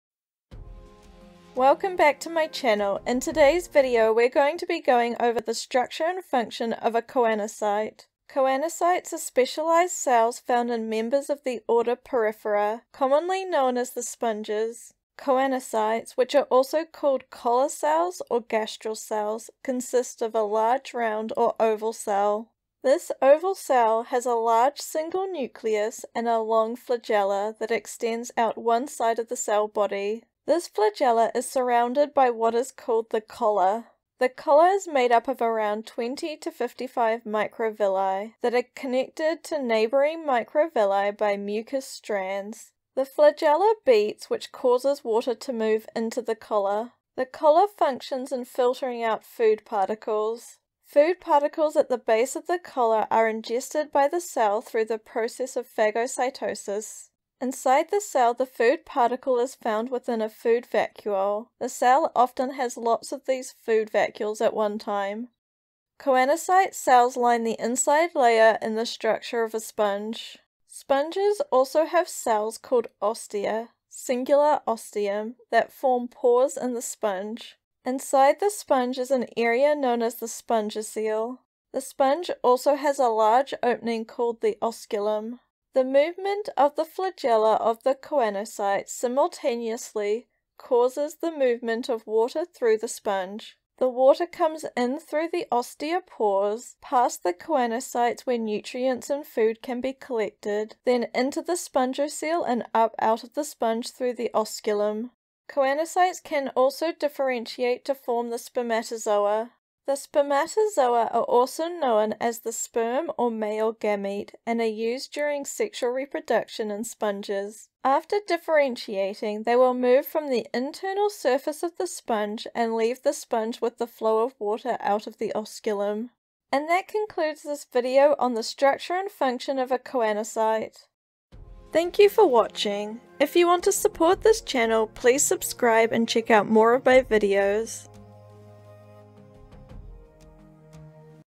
Choanocyte Structure & Function | Invertebrate Biology
In this video, we go over the structure and function of the specialised sponge cell known as a choanocyte.